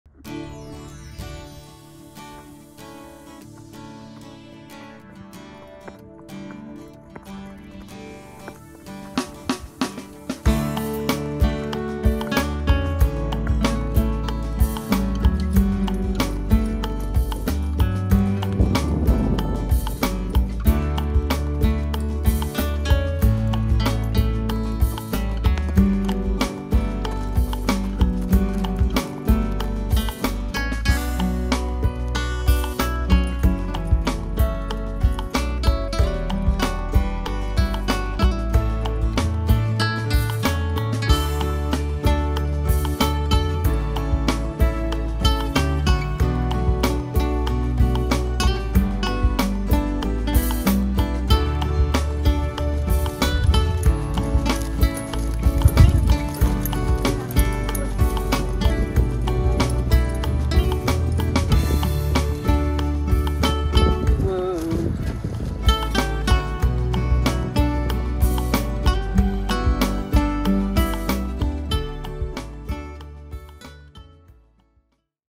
Ranae and Max good Stop 3 2024
Ranae has been working with her horse, Max, on a better stop. She hopes to develop the stop from just her seat. It was a beautiful day on the Preserve here in Central California, so we sprinkled the work in-between a very pleasant ride.